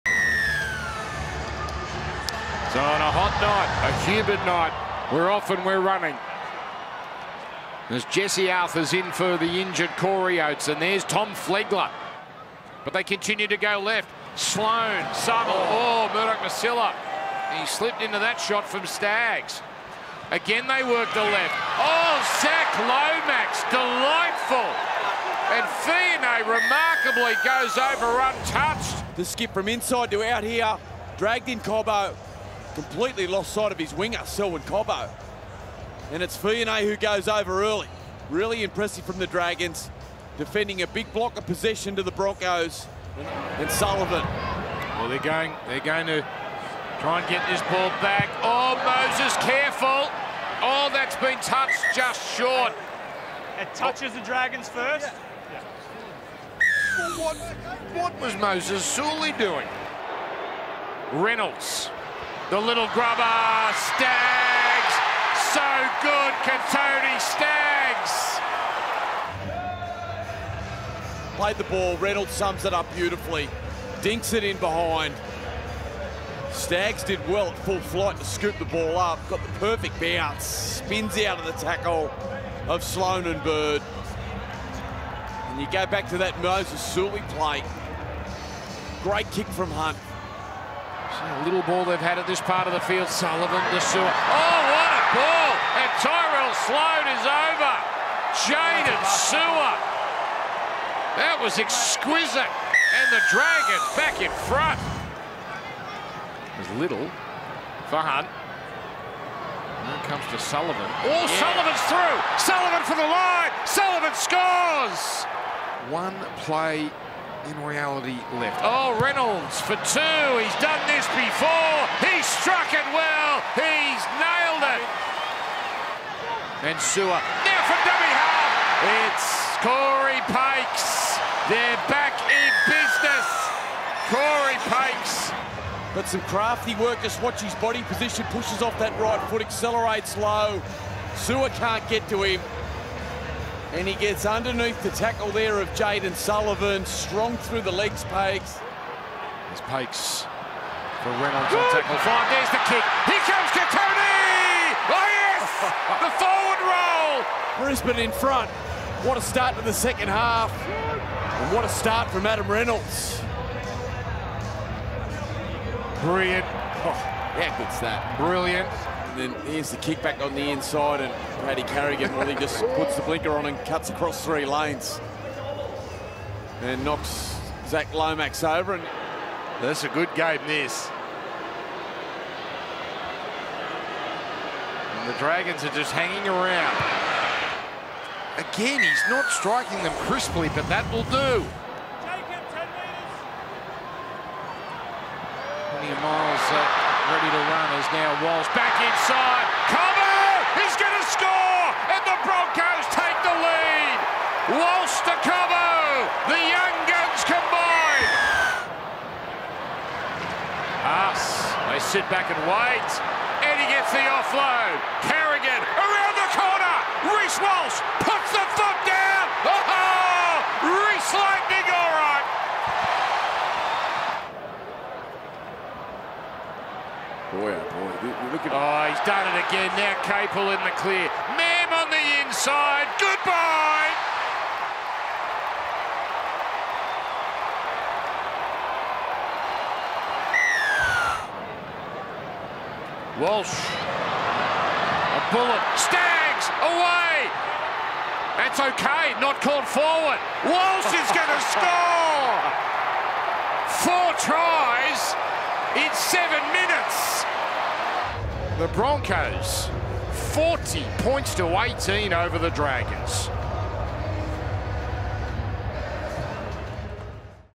NRL 2023 | Broncos v Dragons | Match Highlights
The Brisbane Broncos host the St George Illawarra Dragons during the 2023 NRL Telstra Premiership